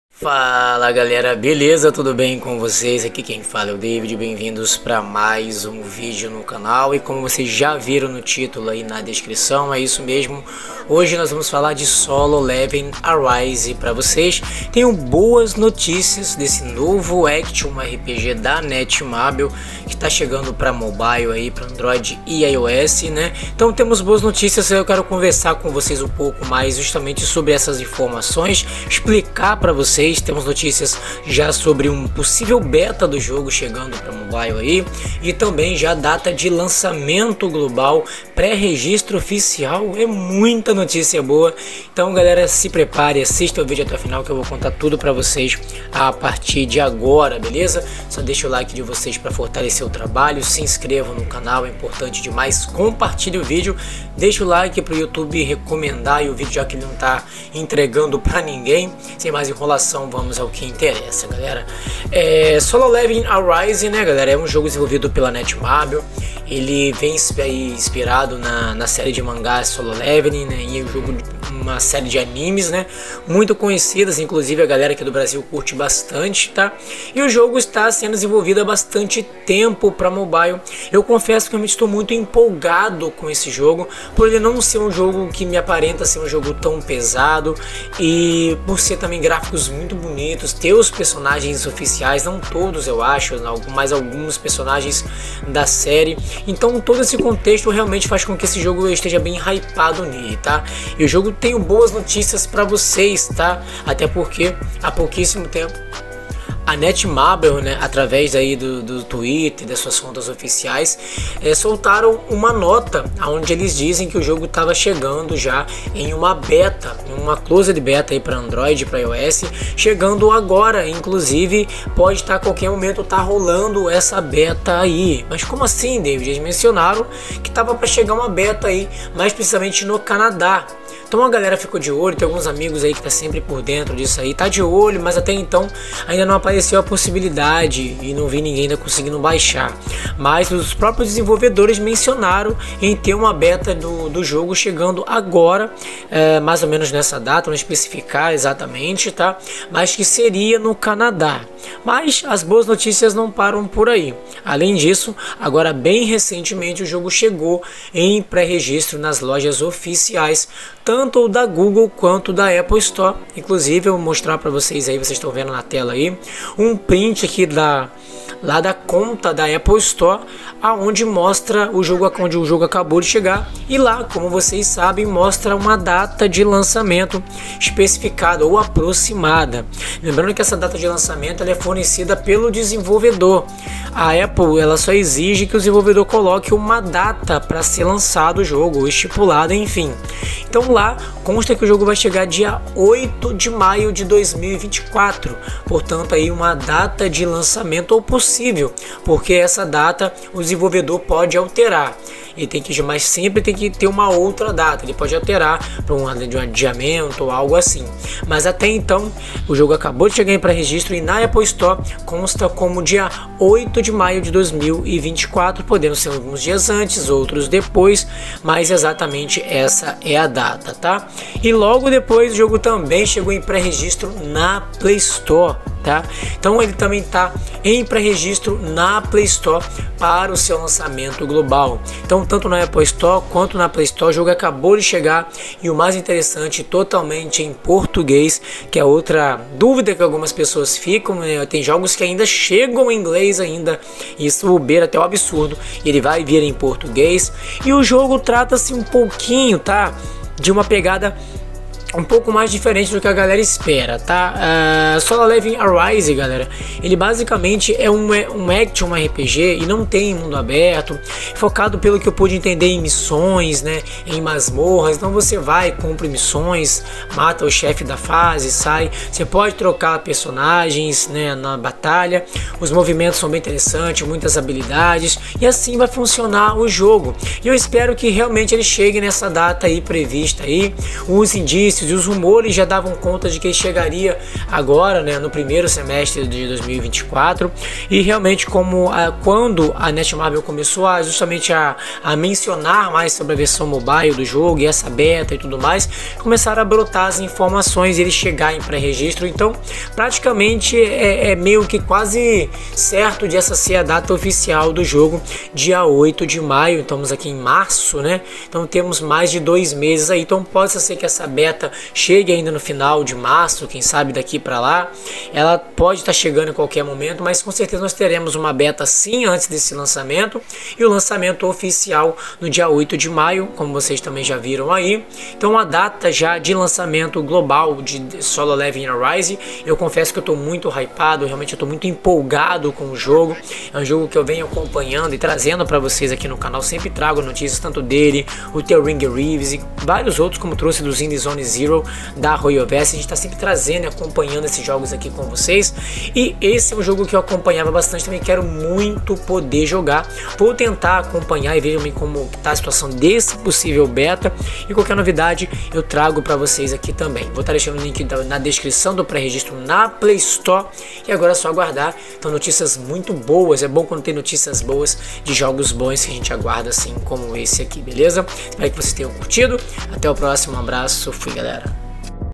SOLO LEVELING ARISE FINALMENTE DATA DE LANÇAMENTO REVELADA + PRE REGISTRO OFICIAL E MUITO MAIS!!
solo leveling ARISE.e um incrível RPG de açao para PC e mobile chegando em 2024 e neste vídeo vamos falar sobre a data de lançamento global e ainda o pre registro oficial na play store idioma e muito mais